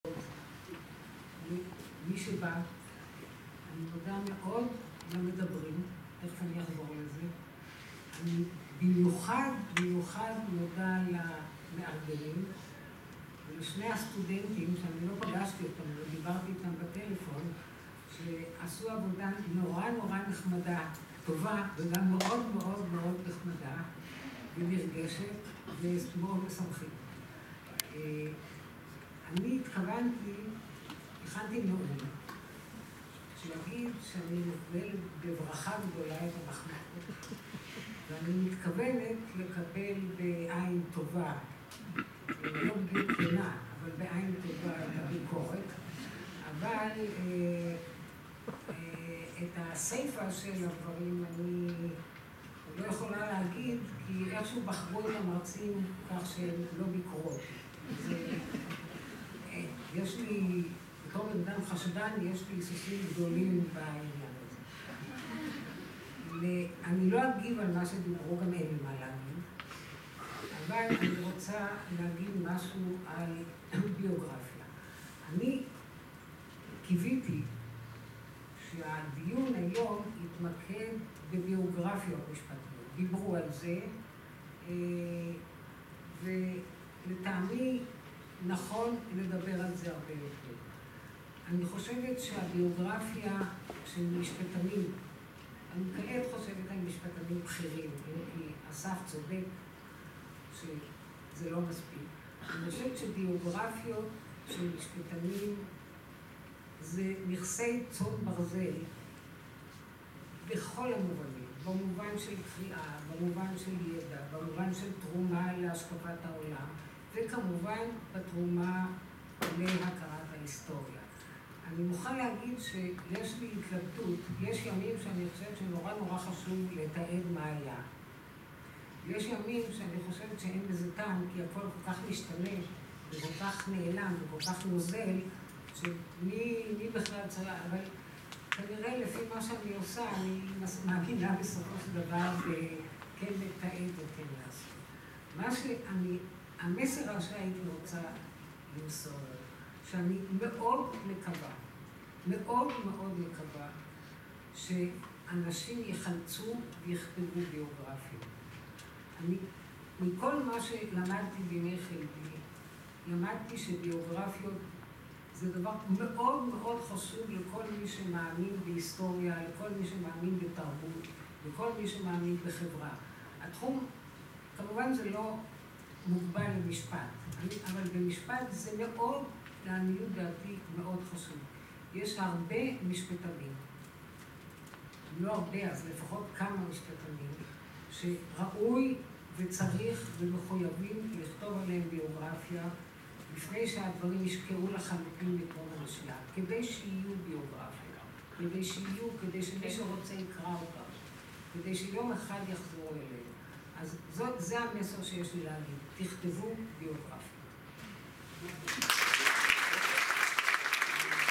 חשיבות כתיבת ביוגרפיות I ד"ר עו"ד מיכל שקד I ערב לכבוד ספרה של מיכל שקד "הראשונה: דורית ביניש"
ד"ר עו"ד מיכל שקד
ערב לכבוד ספרה של מיכל שקד "הראשונה: דורית ביניש"
"פורום עיוני משפט"
מרכז מינרבה לזכויות האדם
המכון למשפט והיסטוריה ע"ש קרן דיויד ברג I הפקולטה למשפטים ע״ש בוכמן I אוניברסיטת תל אביב